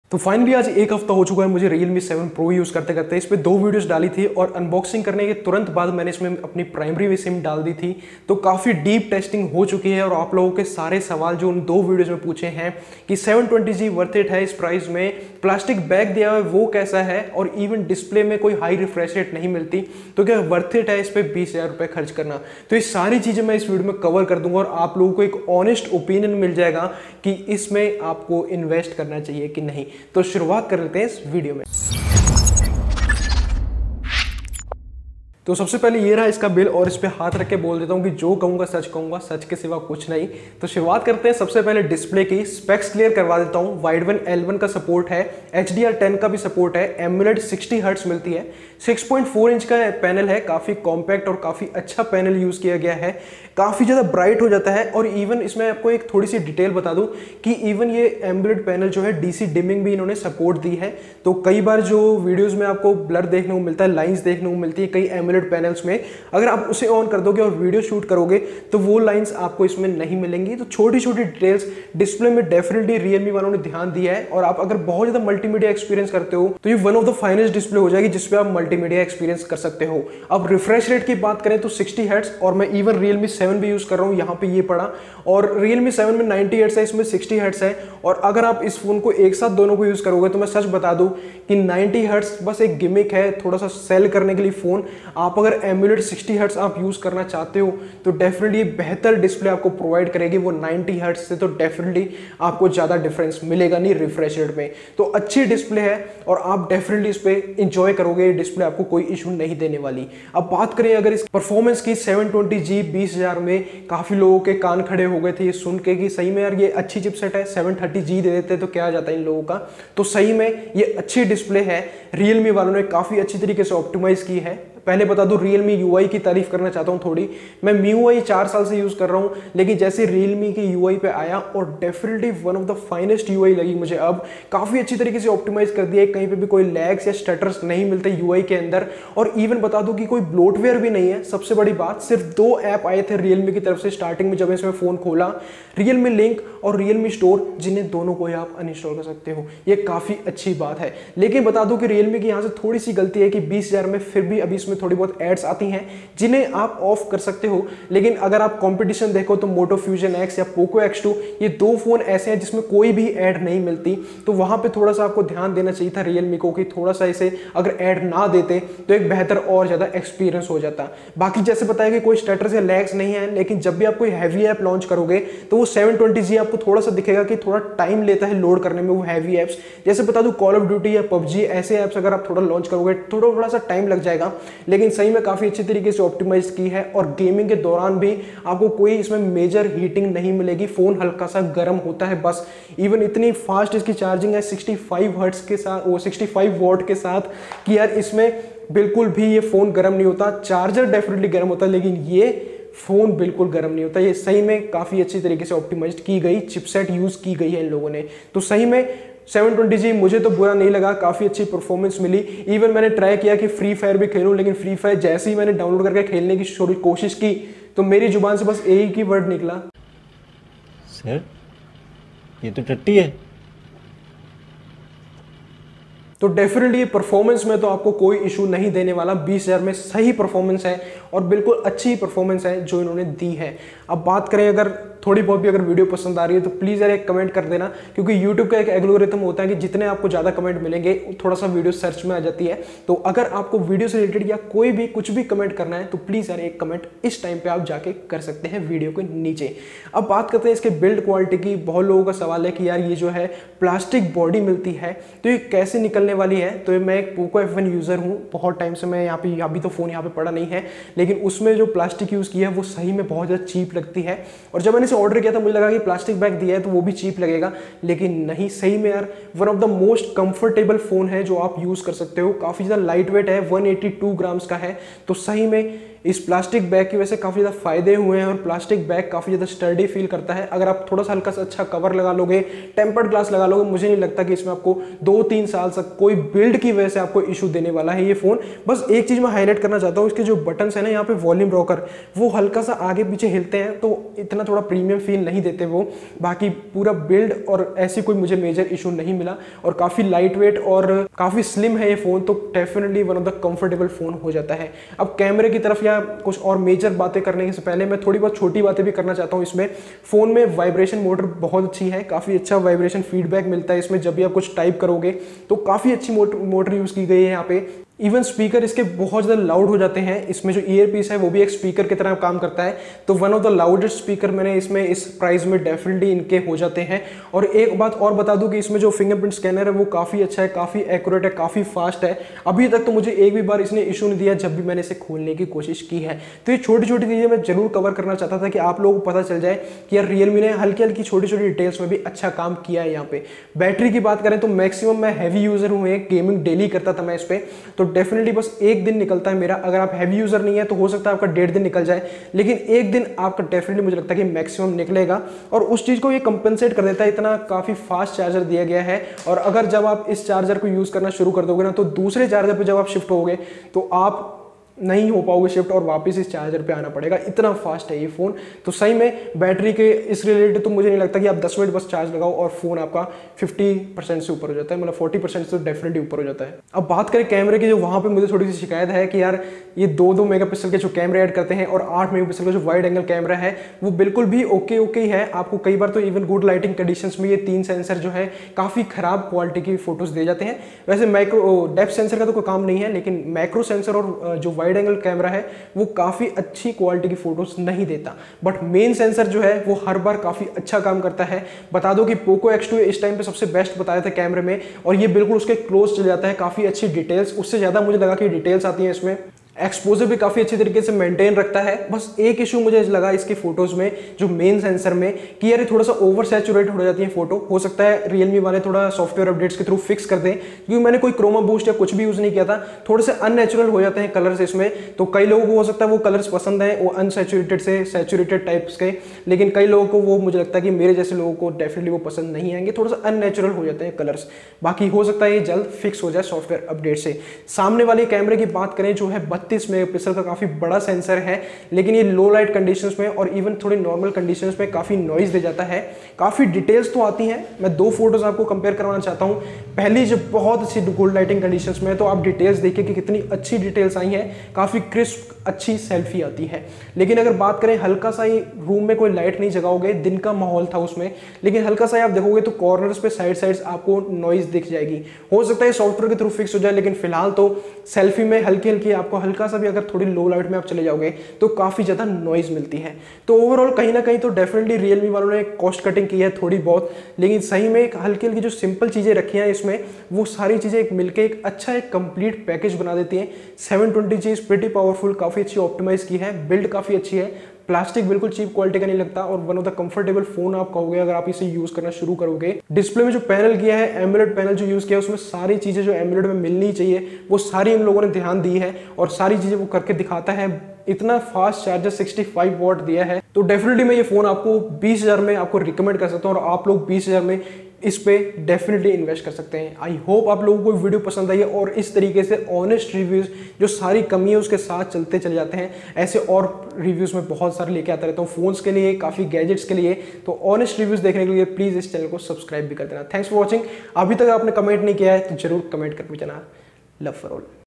Realme 7 Pro After 1 Week Of Usage || IN DEPTH HONEST REVIEW
So is video mein mene complete review kiya hai Realme 7 Pro ka so i hope apko yeh video pasand aye